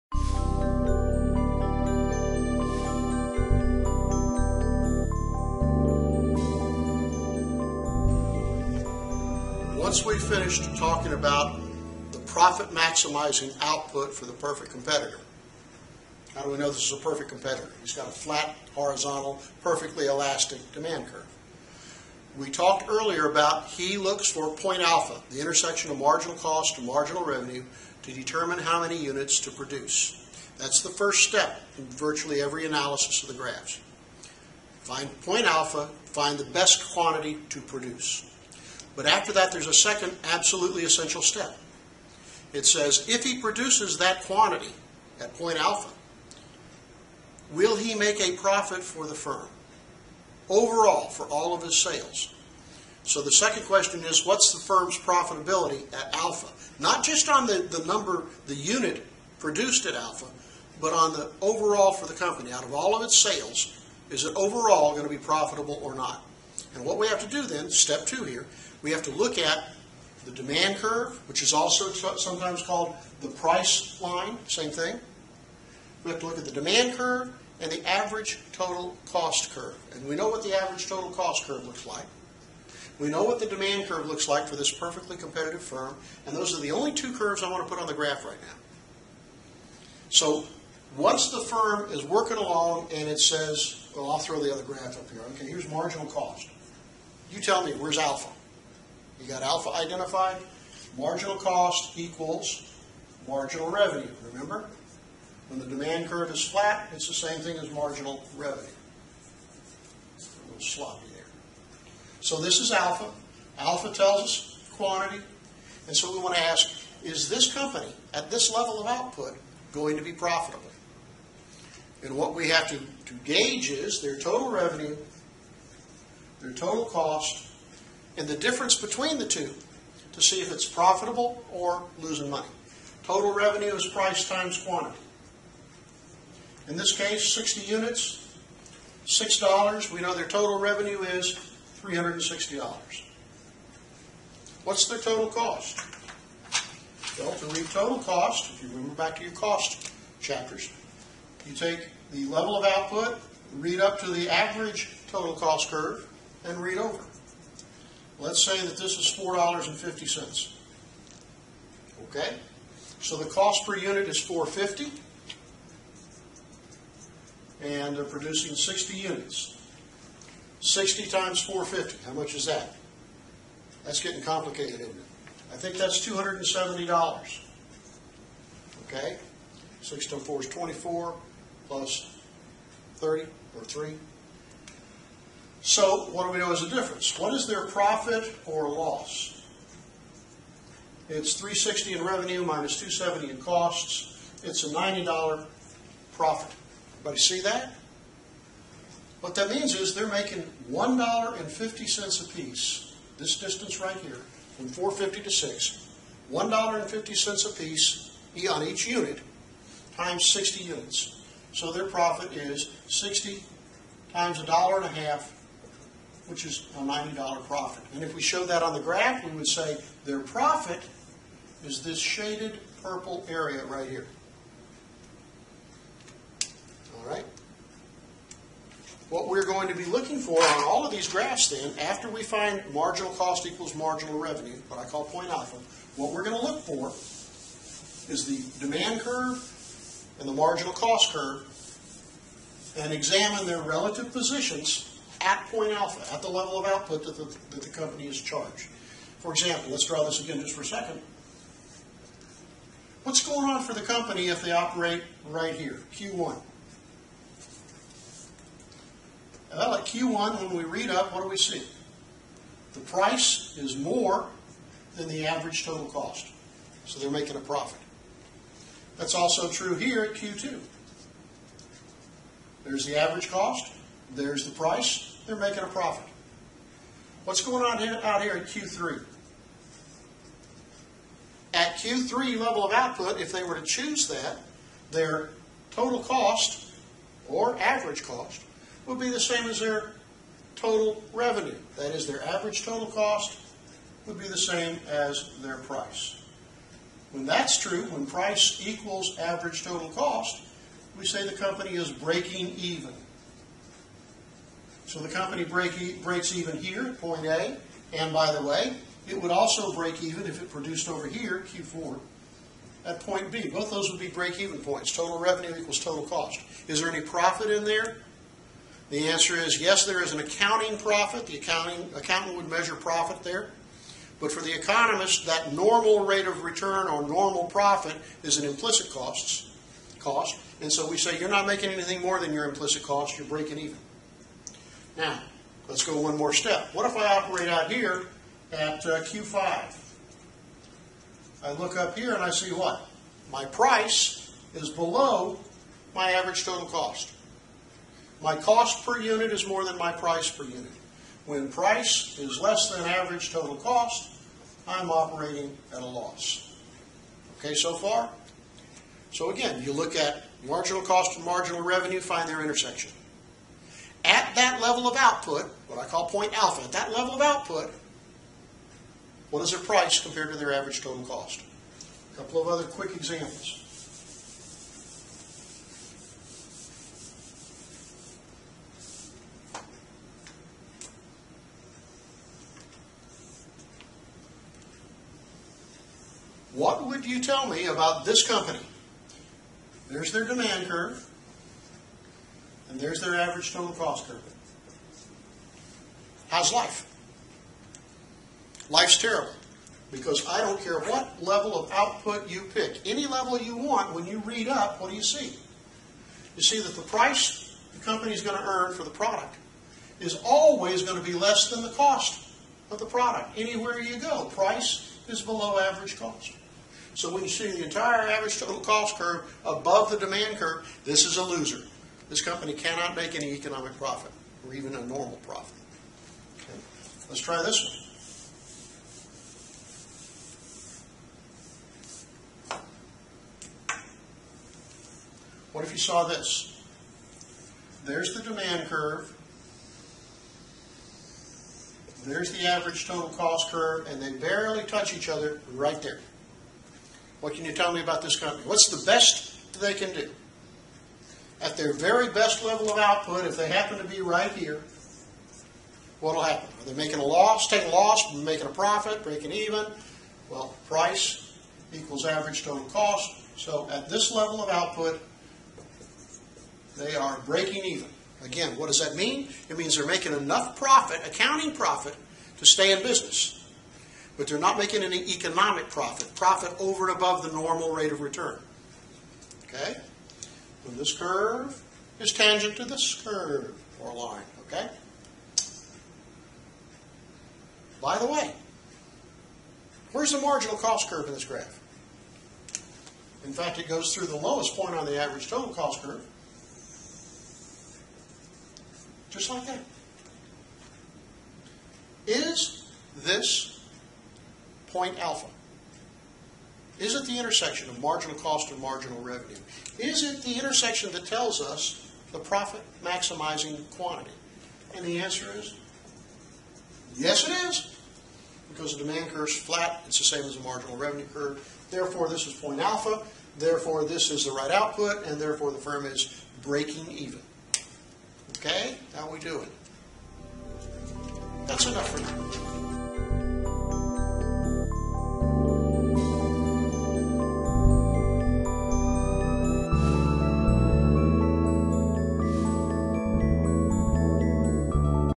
Perfect Competition (4) - Profit or Loss
Determining profit or loss graphically.